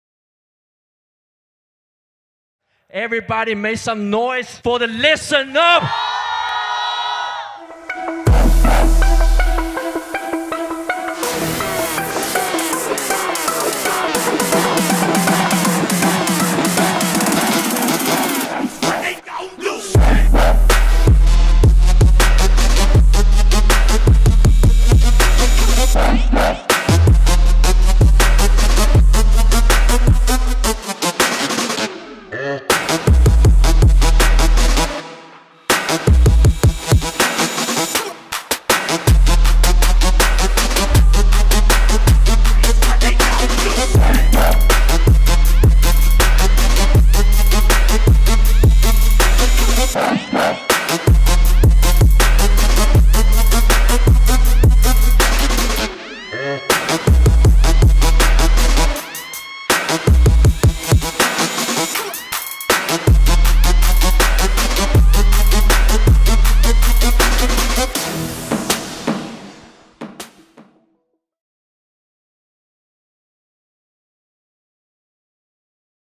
2017ListenUp 說唱歌曲創作大賽 上海站 精華回顧
LISTEN UP不僅僅是一個說唱音樂比賽
它更是一個說唱音樂創作的推動系統，一個旨在支持和發展華語說唱事業的特別計劃。